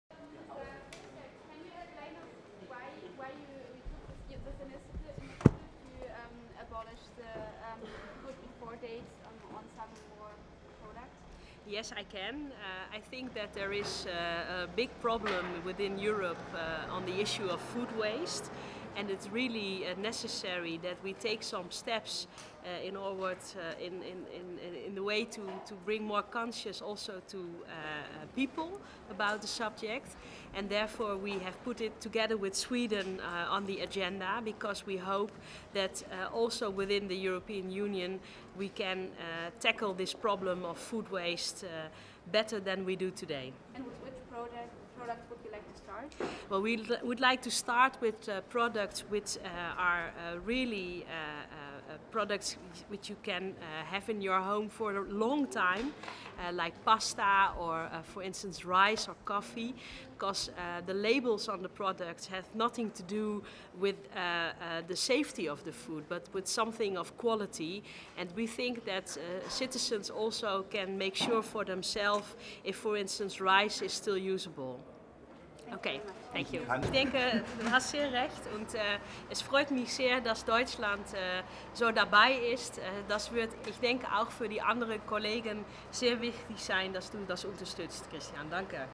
Arrival and doorstep NL (Dijksma)
Arrival and doorstep by Sharon DIJKSMA, Minister for Agriculture of the Netherlands, before the Agriculture and Fisheries Council, taking place on 19 May 2014 in Brussels.